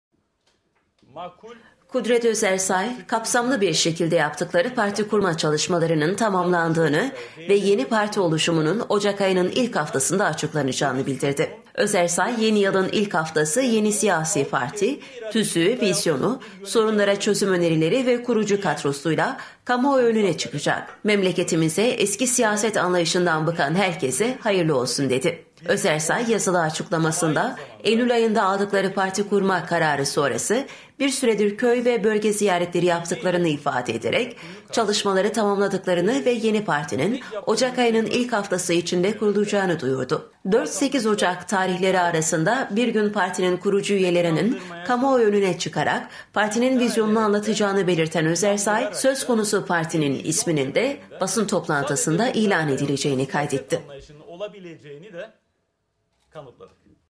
ÖZERSAY: “YENİ PARTİ OCAK’TA KURULUYOR” KUDRET ÖZERSAY
Kudret Özersay, kapsamlı bir şekilde yaptıkları parti kurma çalışmalarının tamamlandığını ve yeni parti oluşumunun Ocak ayının ilk haftasında açıklanacağını bildirdi.  Özersay, “Yeni yılın ilk haftası yeni siyasi parti, tüzüğü, vizyonu, sorunlara çözüm önerileri ve kurucu kadrosuyla kamuoyu önüne çıkacak. Memleketimize, eski siyaset anlayışından bıkan herkese hayırlı olsun” dedi.  Kudret Özersay, yazılı açıklamasında, Eylül ayında aldıkları parti kurma kararı sonrası bir süredir köy ve bölge ziyaretleri yaptıklarını ifade ederek, çalışmaları tamamladıklarını ve yeni partinin Ocak ayının ilk haftası içinde kurulacağını duyurdu.  4-8 Ocak tarihleri arasında bir gün partinin kurucu üyelerinin kamuoyu önüne çıkarak, partinin vizyonunu anlatacağını belirten Özersay, partinin isminin de basın toplantısında ilan edileceğini kaydetti.